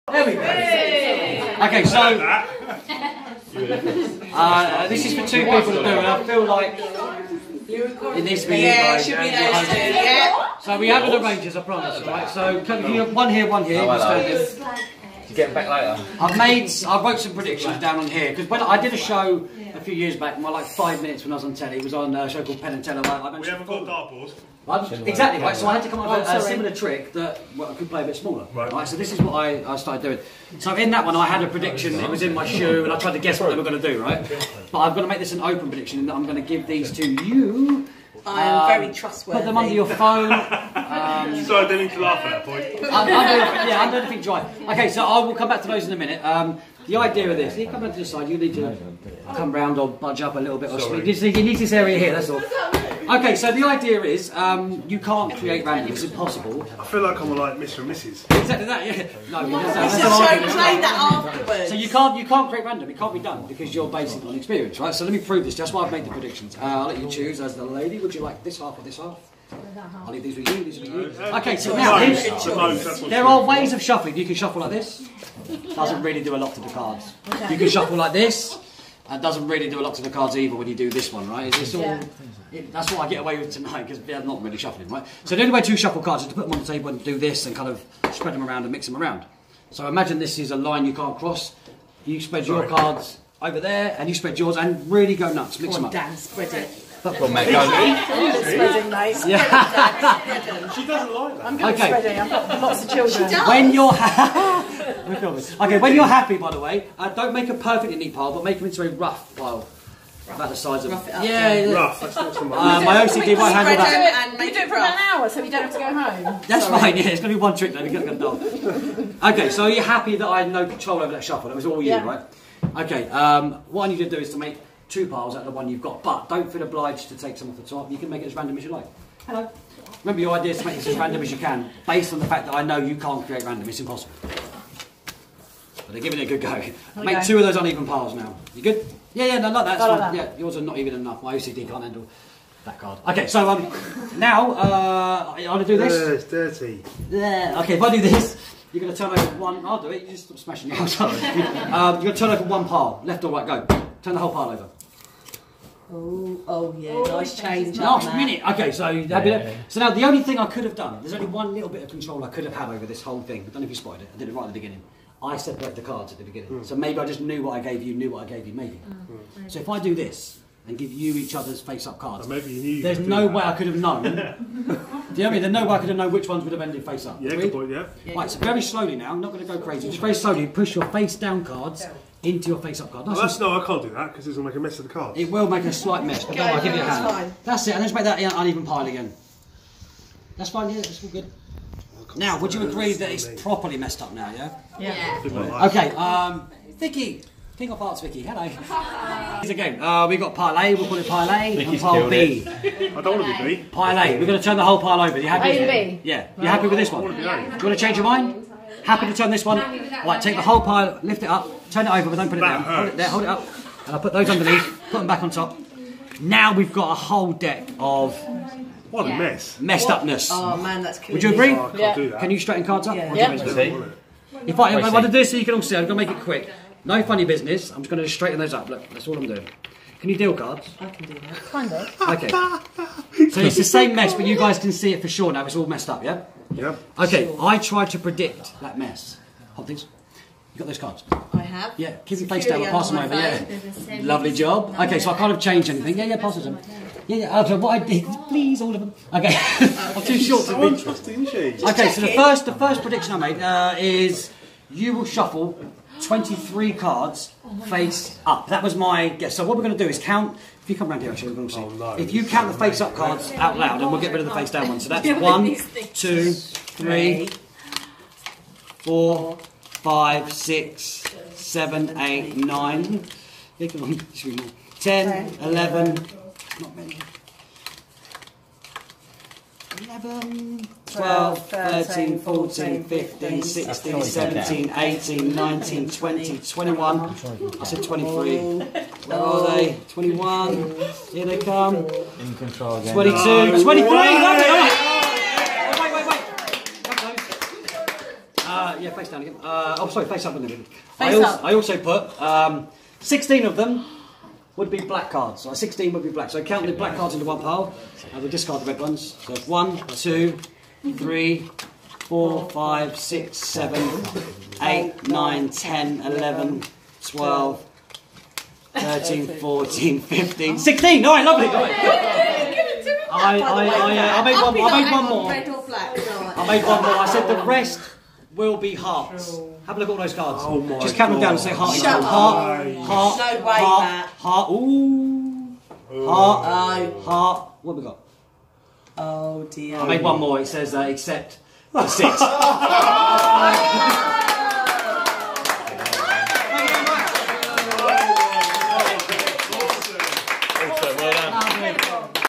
RAW footage from a gig
My favourite closer of a close up set. Sound isn't great. Filming isn't great. But the vibe is good.  Filmed by a spectator called Sam. Cheers Sam